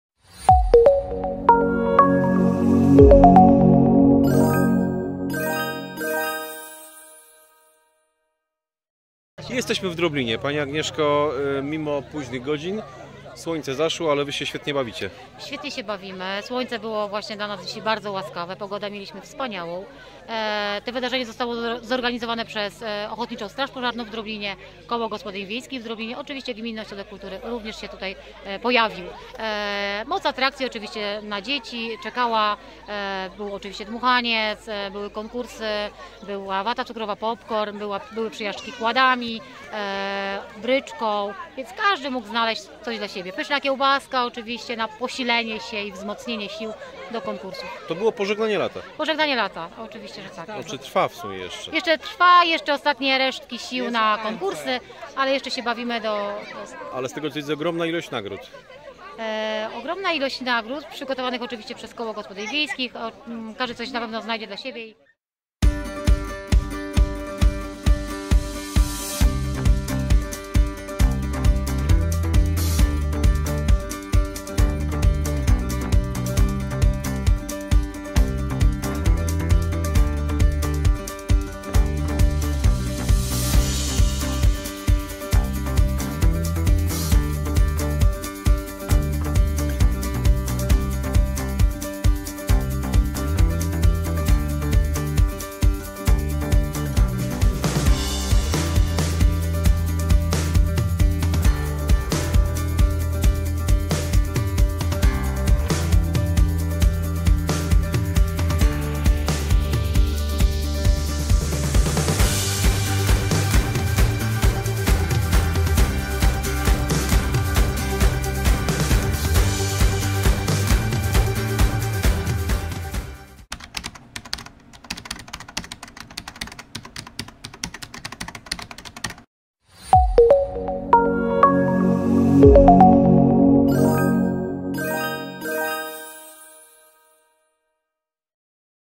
Pożegnanie lata w Droblinie| Radio Biper
25 września Gminny Ośrodek Kultury w Leśnej Podlaskiej, OSP Droblin oraz Samodzielne Koło Gospodyń Wiejskich w Droblinie zaprosili do Droblina na imprezę z okazji pożegnania lata.